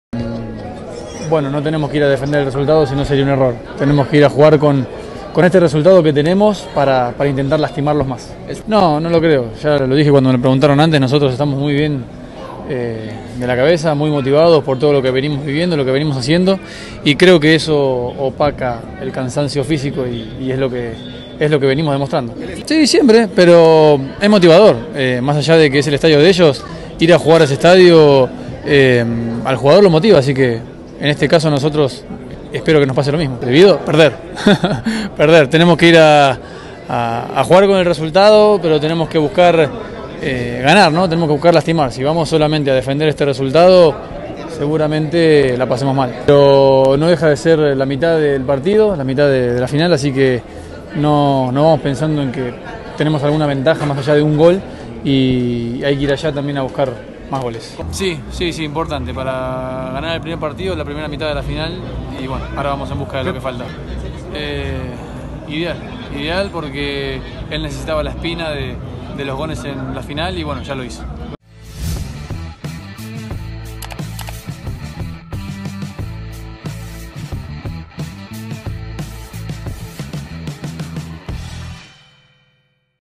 Final | Está prohibido perder en el Azteca | ONCEDiario
Nicolás Sánchez está satisfecho con la victoria en el BBVA pero quieren ir por más. Está prohibido perder.

📹: Javier Alonso